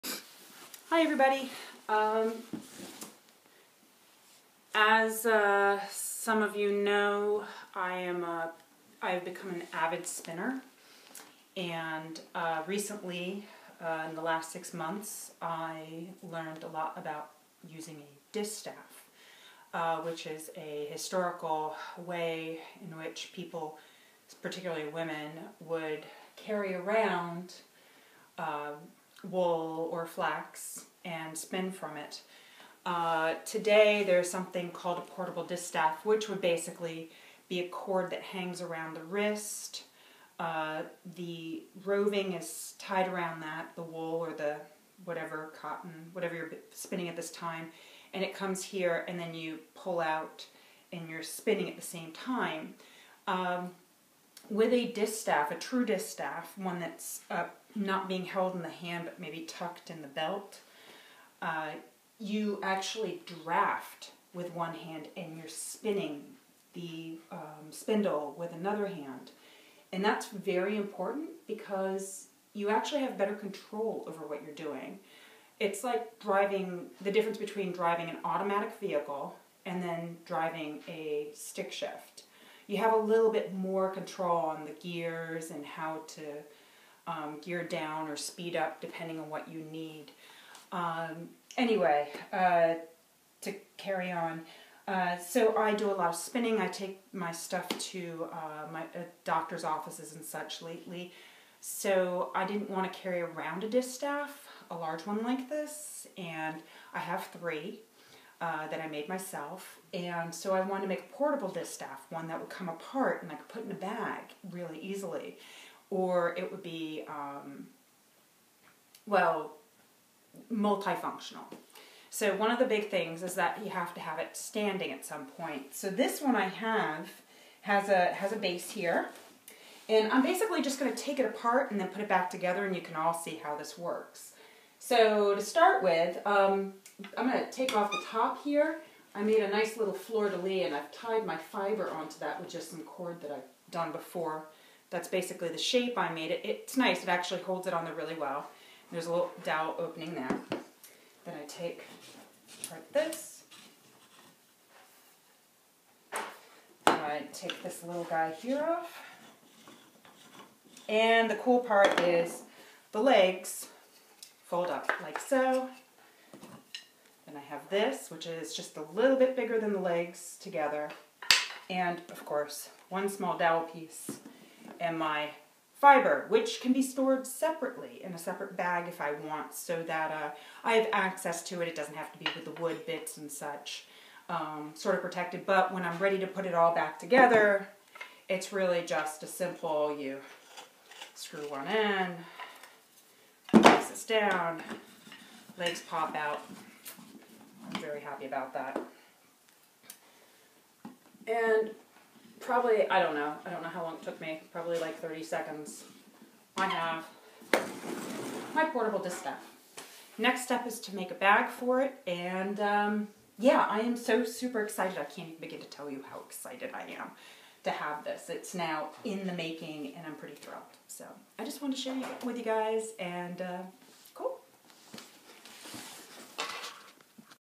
Portable, breakdown distaff. Used for spinning wool or flax. This is an original idea, by Barry! me!
Distaff that can be taken apart for easy portability. This is a design I've worked on over the last few weeks. Enjoy for all you spinners of fiber!